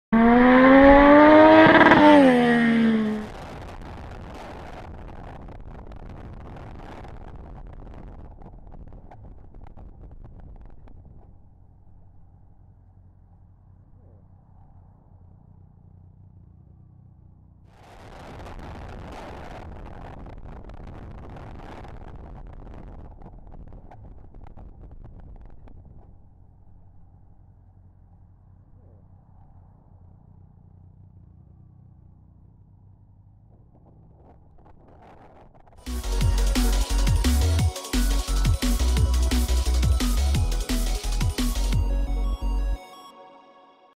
Wymusił pierwszeństwo na motocykliście - Fabiq Moto S16EP30
Czy ja ostatnio jestem przeźroczysty czy co? Jak tak to mrugają, że na długich światłach jadę, a głośny tłumik jest za głośny ale na skrzyżowaniu to już mnie nie widzą?!